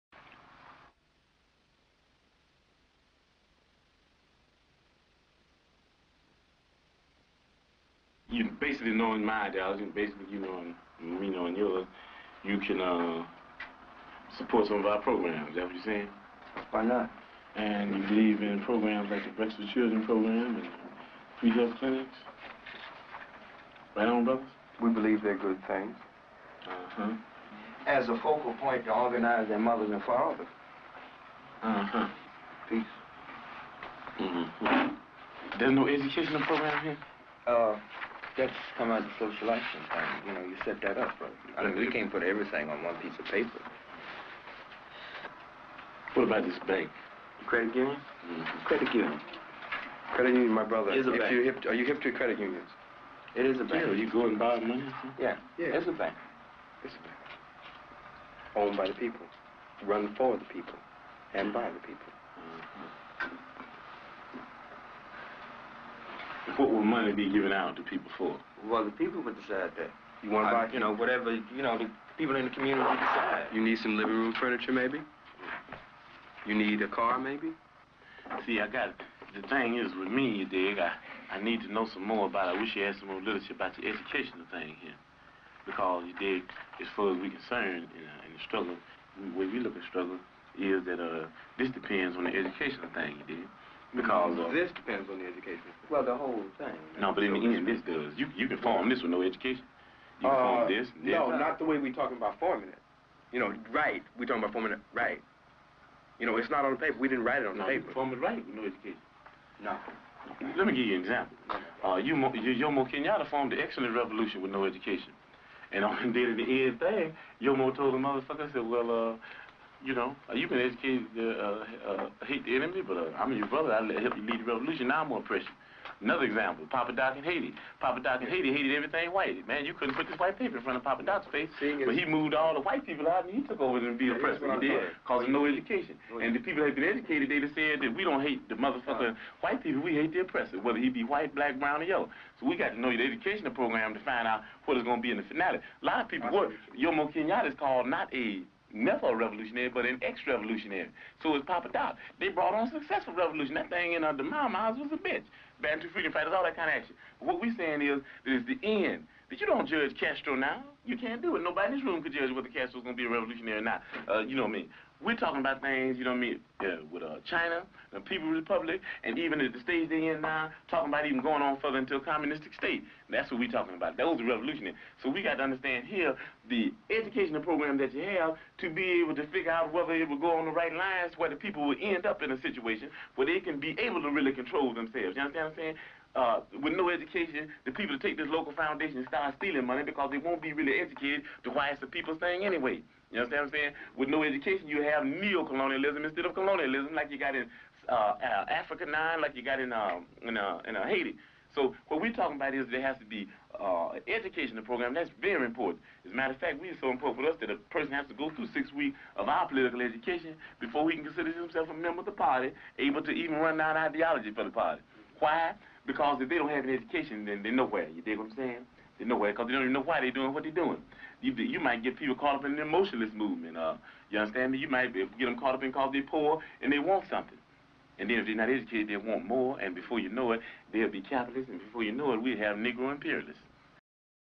Fred Hampton - On The Importance Of Education Prior To Action
Hampton founded the Chicago chapter of the Black Panther Party in November 1968 where he immediately established a community service program. This included the provision of free breakfasts for schoolchildren and a medical clinic that did not charge patients for treatment. Hampton also taught political education classes and instigated a community control of police.

In the early hours of the 4th December, 1969, the Panther headquarters was raided by the police for the fourth time. The police later claimed that the Panthers opened fire and a shoot-out took place. During the next ten minutes Fred Hampton and Mark Clark were killed. Witnesses claimed that Hampton was wounded in the shoulder and then executed by a shot to the head.